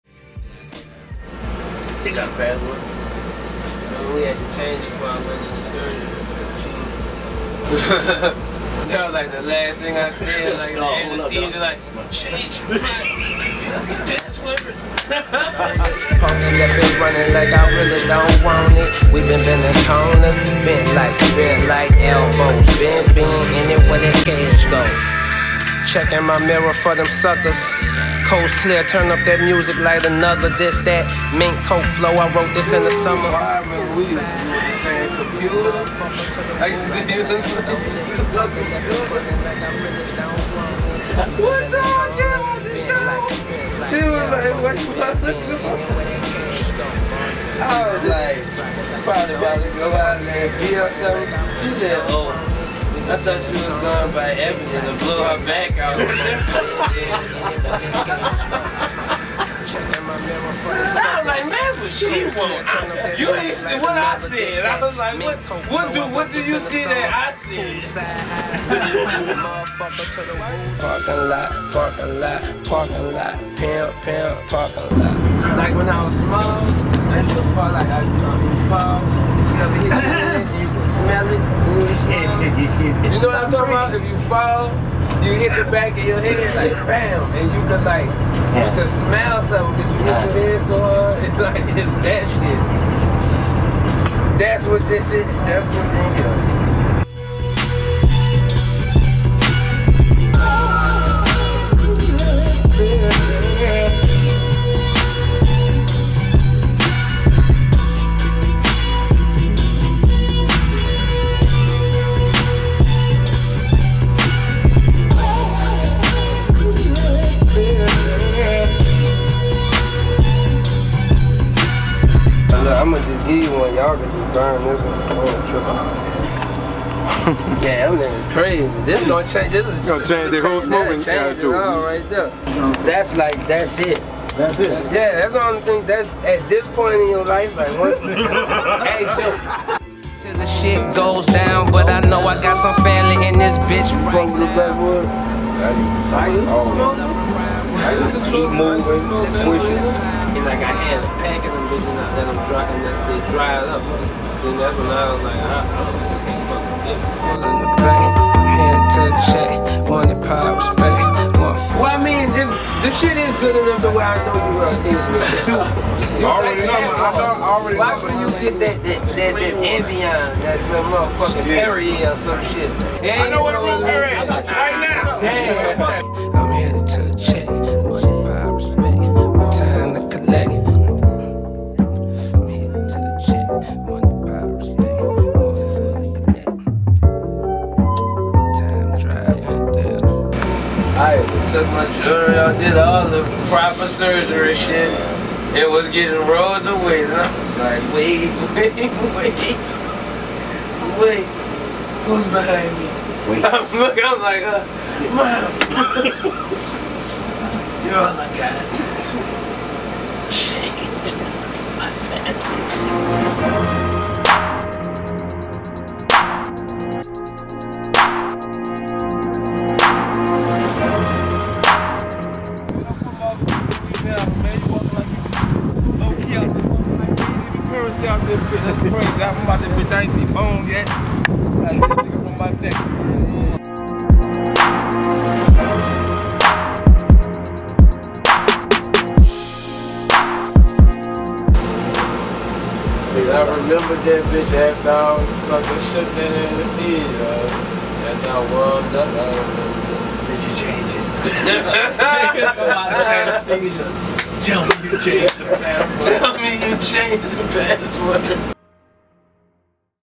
Spitta Andretti - The Drive In Theatre Tour Episode #3 - BUS LIFE
Visuals provided by Saddi @understream

Spitta Andretti let us take a peek on the Jet Life Bus to get an inside look. Come inside and see whats going on with Curren$y and the rest of the Jet Life Gang as they go from city to city on the Drive In Theatre Tour.

Courtesy of The Never Die Corporation